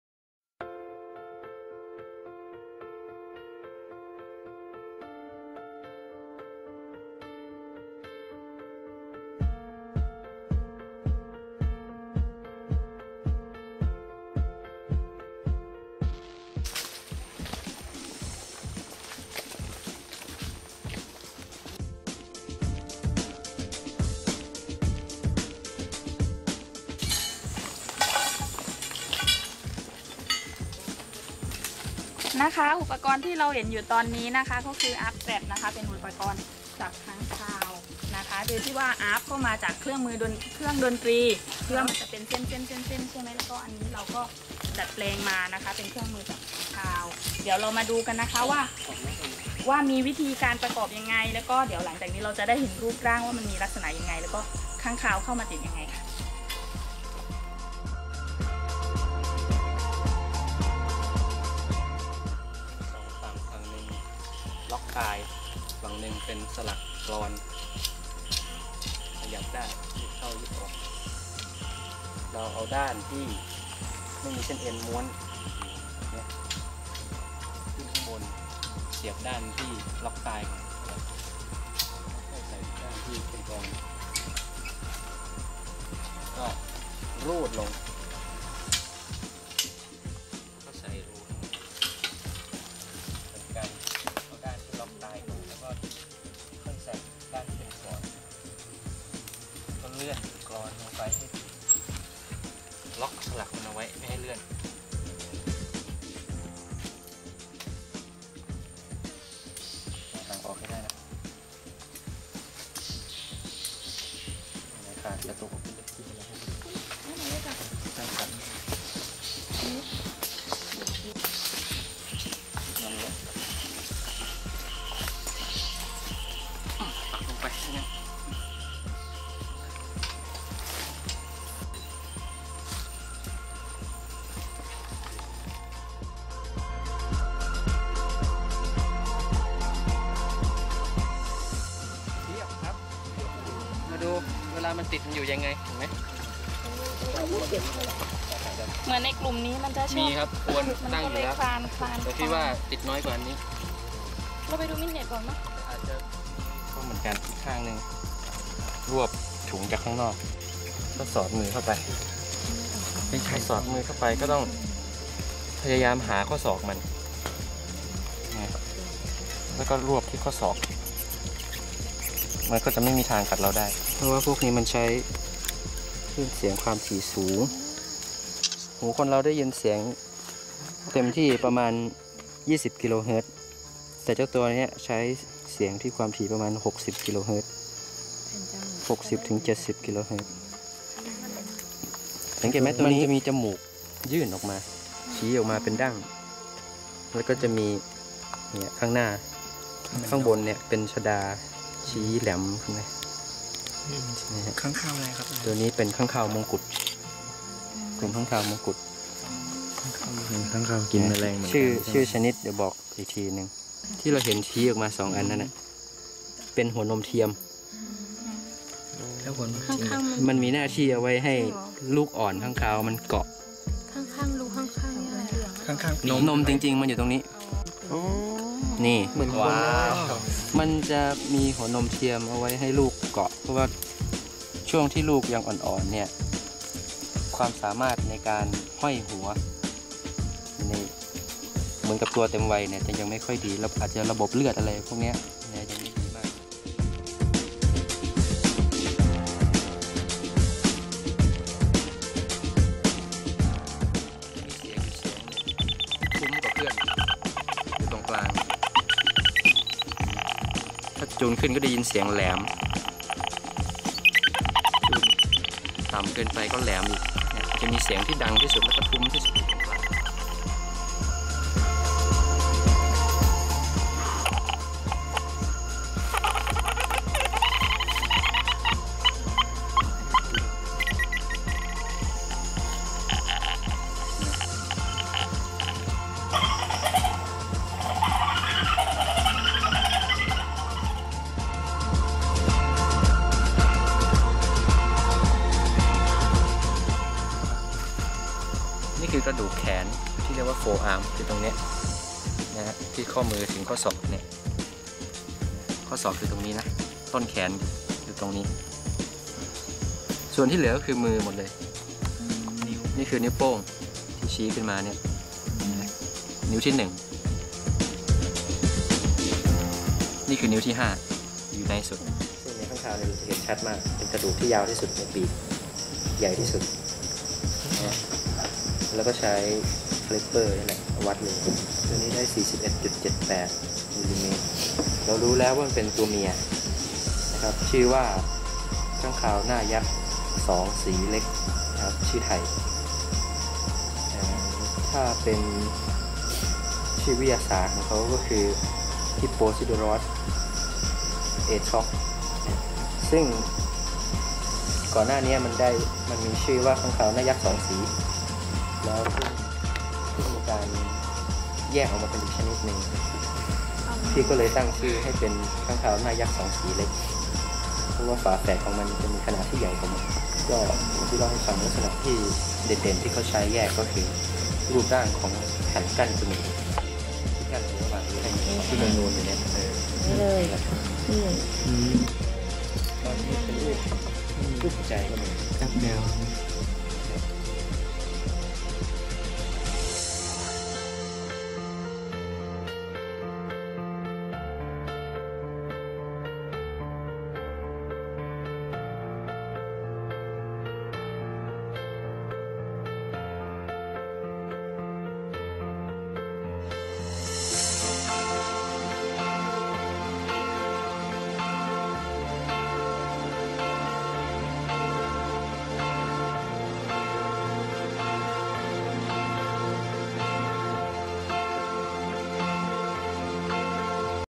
Field Study for Microbat
Microbats or insectivorous bats are very important to tropical ecosystems. They are very diverse and abundant, in particular in Southeast Asia. Here is a methodology to study them.